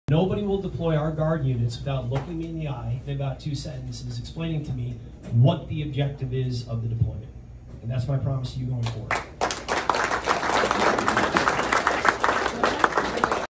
Wisconsin’s National Guard Units
As Governor, I can promise you this: no President will deploy Wisconsin’s National Guard units without looking me in the eye and telling me what the strategic objective of the deployment is.